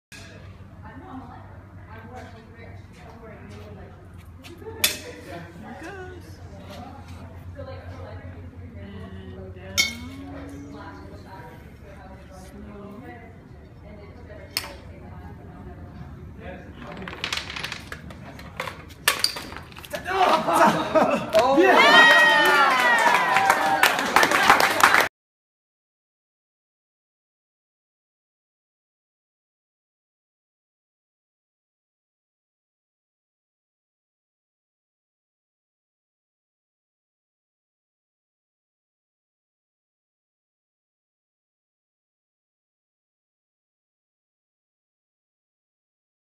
LJCDS AP Physics Rube Goldberg Projects 2015
The Rube Goldberg project in AP Physics 2 allows students to apply their knowledge of physics, energy, and interactions in the creative design and fabrication of a machine in the spirit of those drawn by the 20 th century cartoonist Rube Goldberg.  The students’ contraptions incorporated a series of elaborate interactions (mechanical, electrical, magnetic, optical, using waves and fluids, etc.) to accomplish a task.  This year, one project’s objective was to sink the Titanic (well, at least a model of it)!